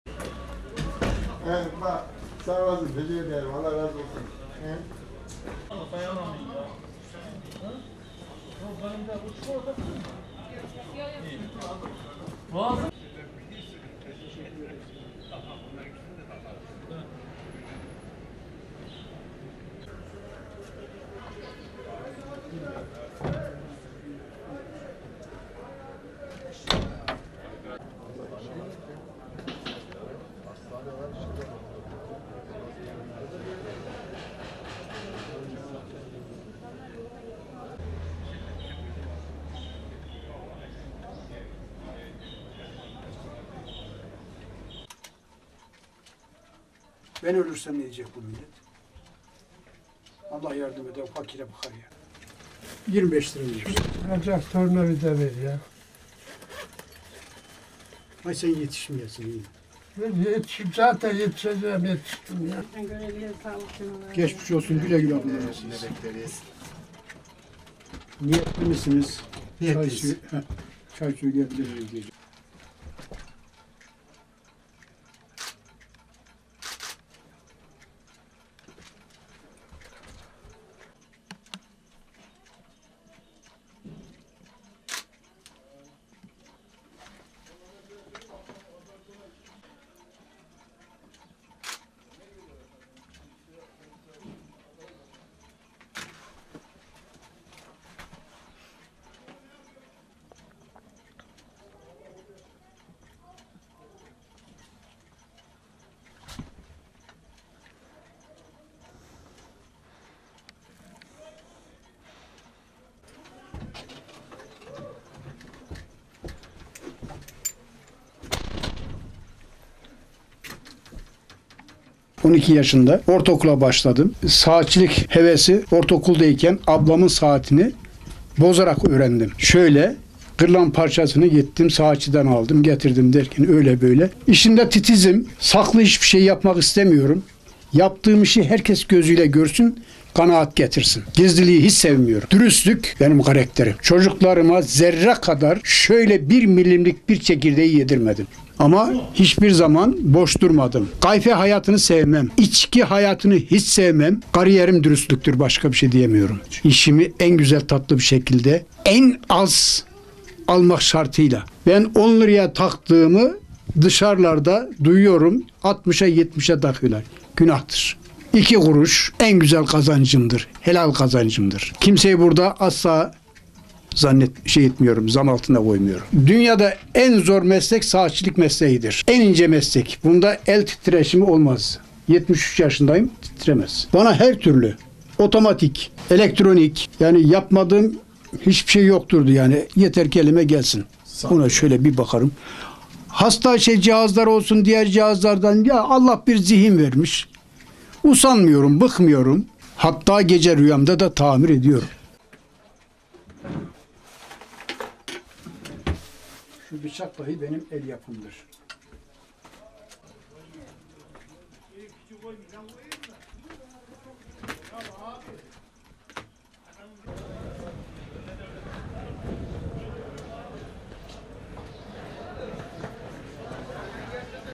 Ablasının Saatini Tamirle Başladığı Meslekte 61 Yılı Geride Bıraktı
Sivas'ın Divriği ilçesinde Cemal Fırat (73), 12 yaşındayken ablası Hatun Erdoğan'ın bozulan saatini yapmaya çalışarak merak sardığı saat tamirciliğinde 61 yılı geride bıraktı. İlçedeki 5 metrekarelik dükkanında analog, dijital ve kurmalı saatleri tamir etmeye devam eden Fırat, "Saat tamirciliği zor ve ince bir meslek" dedi.
Hüsnü Ümit AVCI-Yasin KIRAS/SİVAS, (DHA) -